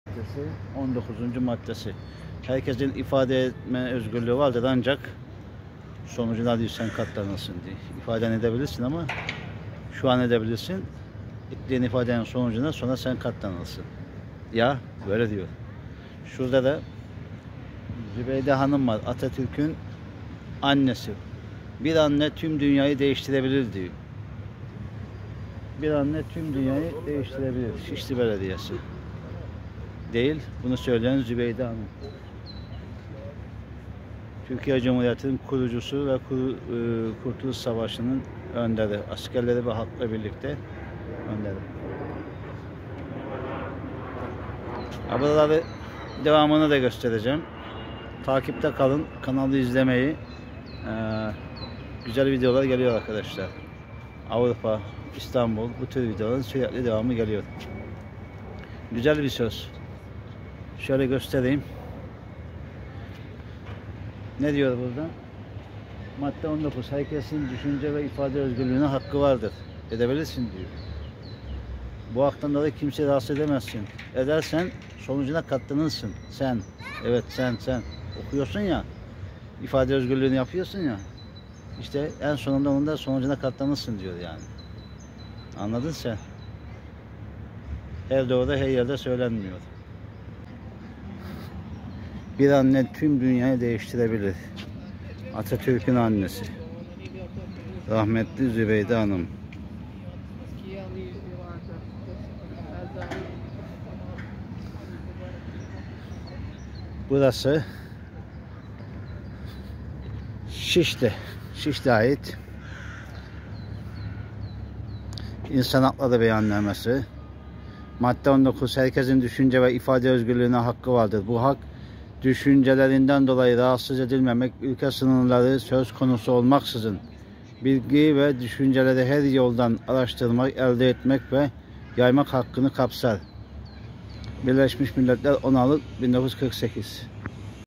Herkesin ifade etme özgürlüğü vardır.ancak. Sonucuna katlanırsın.İnsan hakları beyannamesi.19 Madde
19.Madde. Herkesin ifade etme özgürlüğü vardır. ancak. Sonucuna katlanırsın. İnsan hakları beyannamesi bu şekilde ifade ediyor.. yani konuşursun ,düşünceni belirtirsin ancak ! u düşünceden ve ifaden den dolayı ,konusu suç teşkil ediyorsa yargılanırsın.

      Teşekkürler.